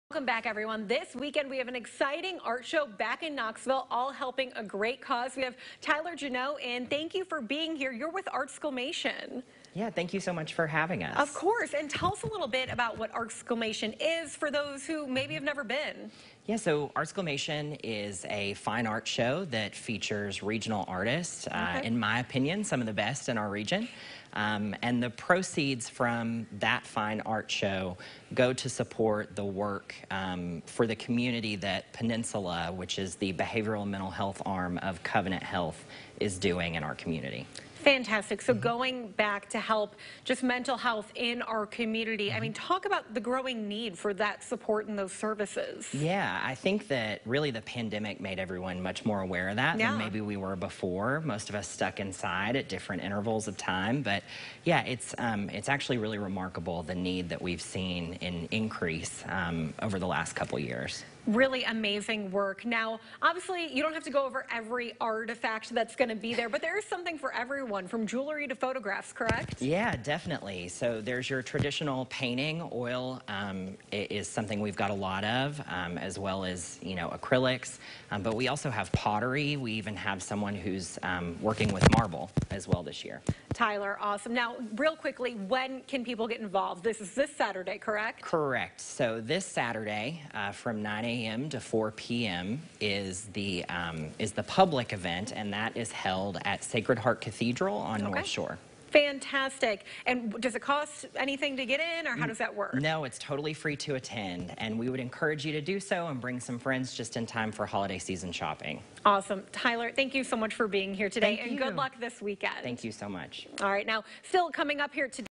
Artsclamation! returning to Knoxville
Artsclamation!, an indoor fine art sale featuring professional regional artists in all mediums, is returnign to Knoxville this weekend. The sale event will be located at Sacred Heart Cathedral in the new Cathedral Hall.